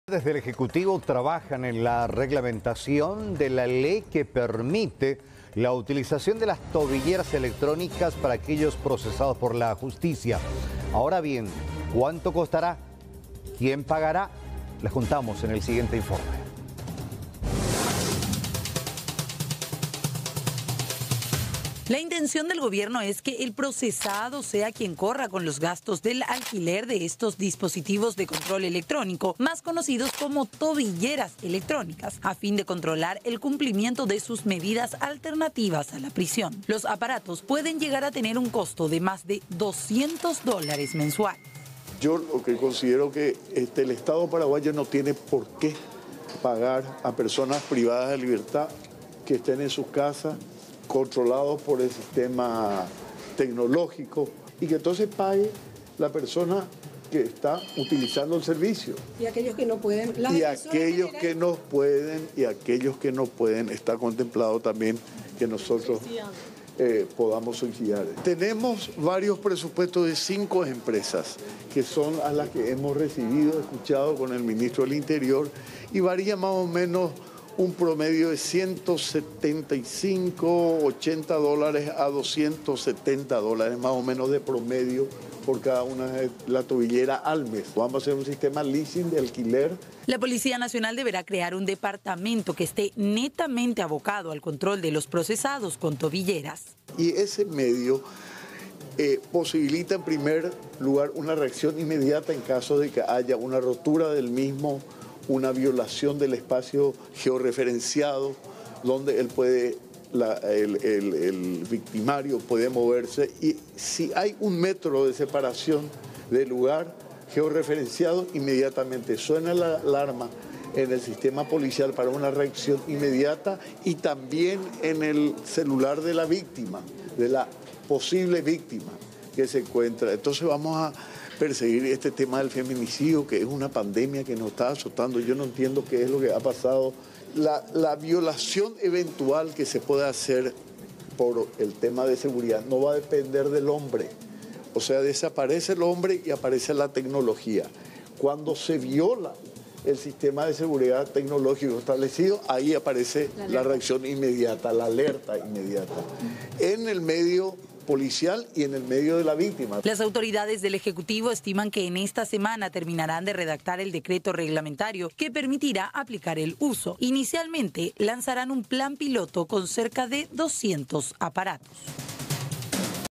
El que use tobillera deberá de pagarla
La intención del gobierno es que el procesado sea quien corra con los gastos del alquiler de estos dispositivos de control electrónico, más conocidos como tobilleras electrónicas.

Los aparatos pueden llegar a tener un costo de más de 200 dólares mensuales.

Igualmente, implicaría la creación de un departamento nuevo en la Policía Nacional.